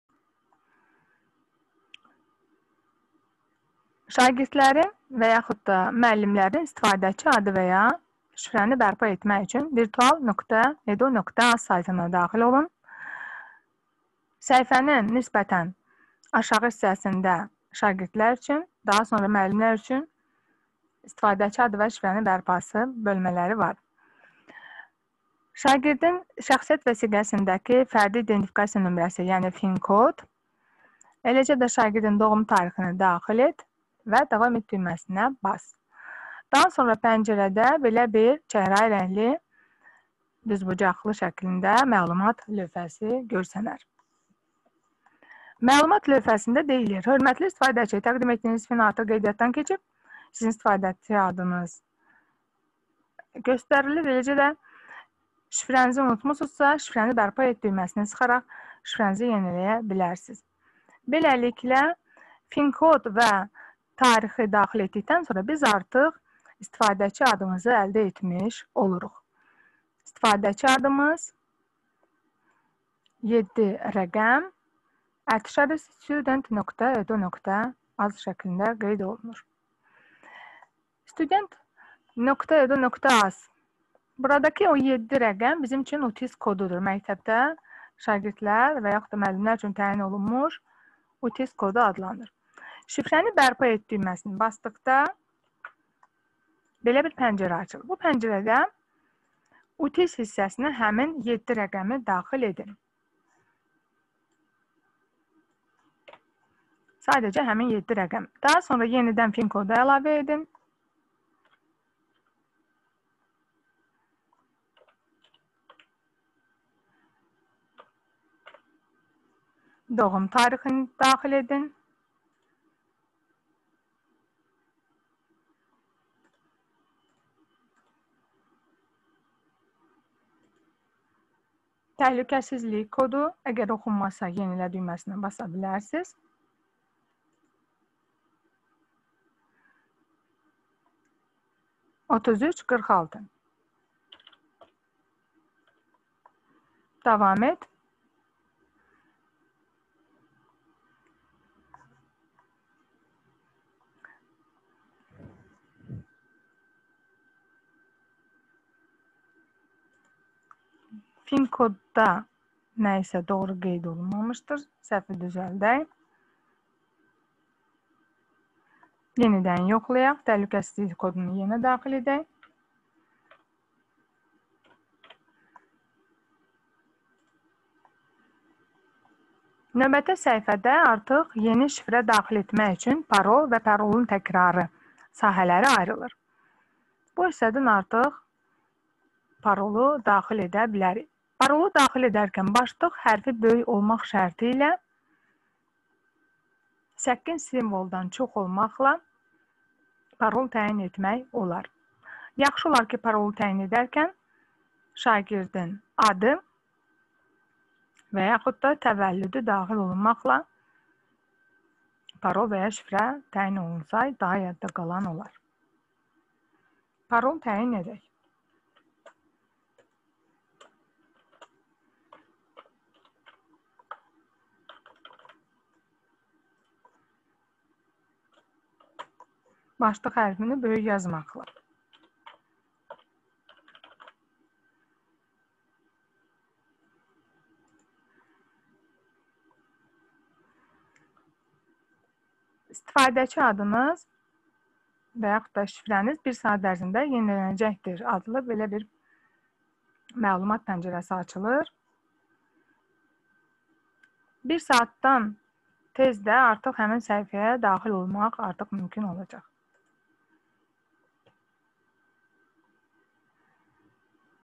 Virtual məktəbə daxil olmaq üçün istifadəçi adı və şifrənin bərpası
Gəncə şəhər C.Cabbarlı adına 6 saylı tam orta məktəb
Səadət Həsənova